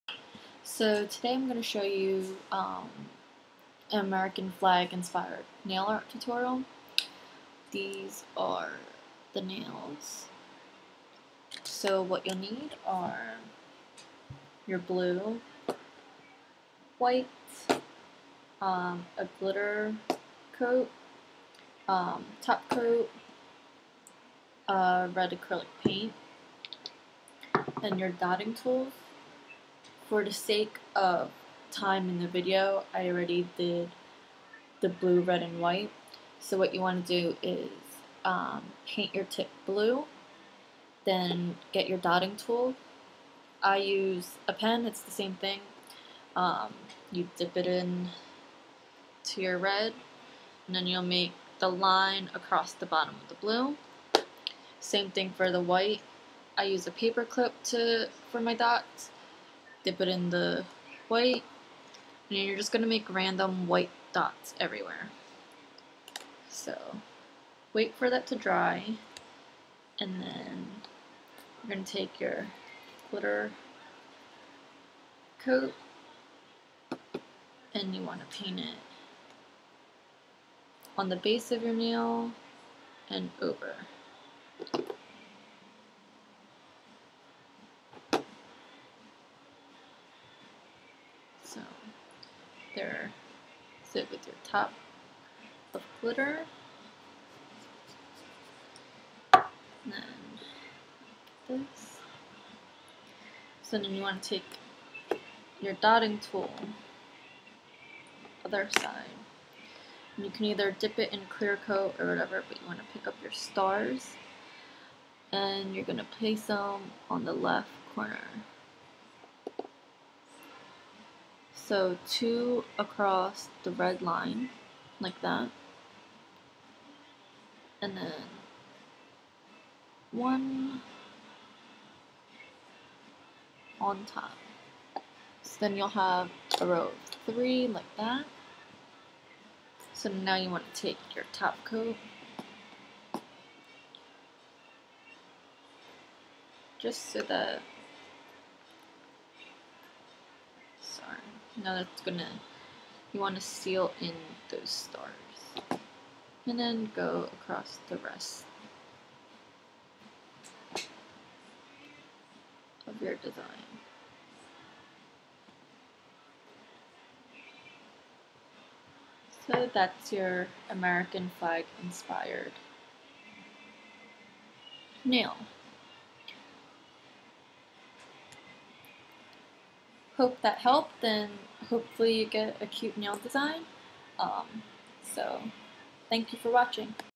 American Flag Inspired nails/nail art tutorial
items used in this video:
sally hansen hard as nails-White on
sally hansen hard as nails-Blue it
Anitas acrylic paint- Christmas red
china glaze glitter polish
konad top coat
silver stars (3 for each nail)